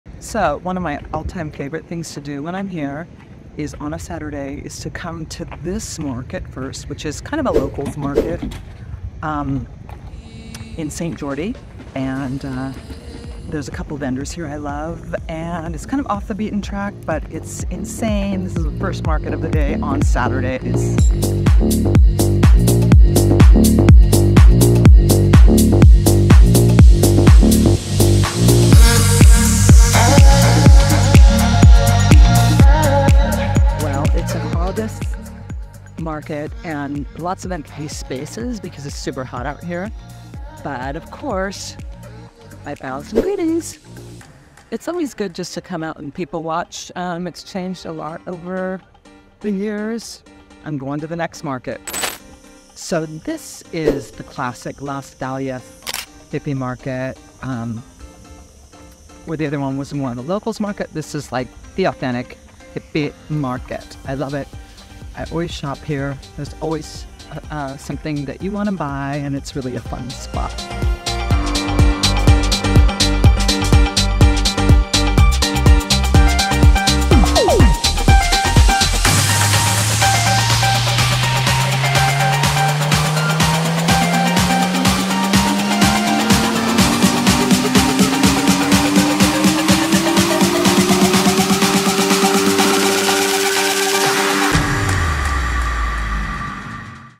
EXPLORING the beauty of IBIZA & HIPPY market
Embark on a captivating journey with me as I explore the multifaceted beauty of Ibiza and delve into the vibrant tapestry of the Hippy Market.